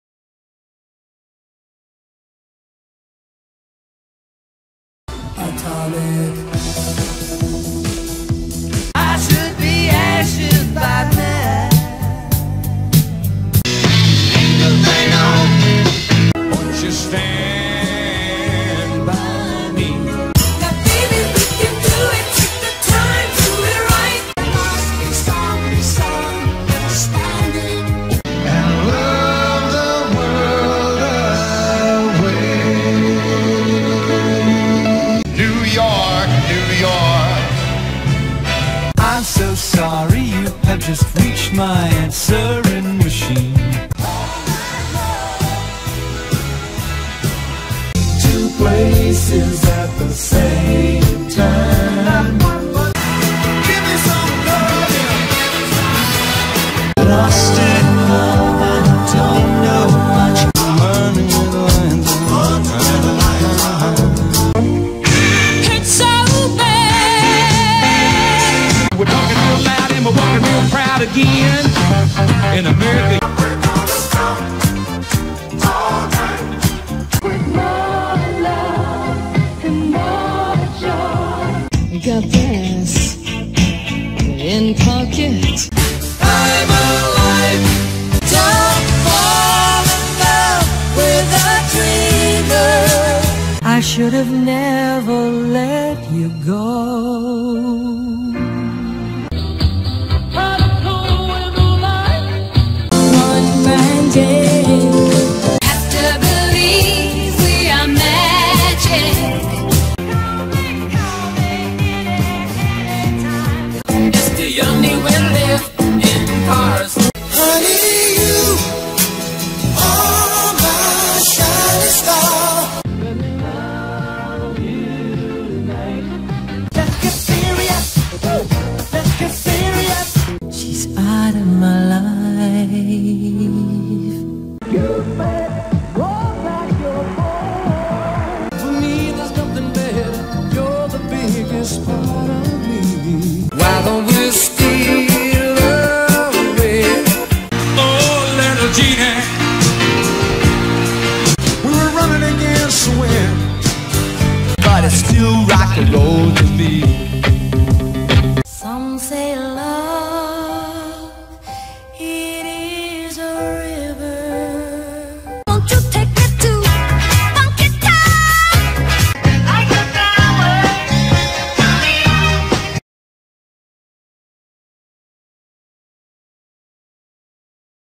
Billboard Top 40 Hits - June 28, 1980
The Top 40 Songs on the Billboard Hot 100 chart for the week ending June 28. 1980.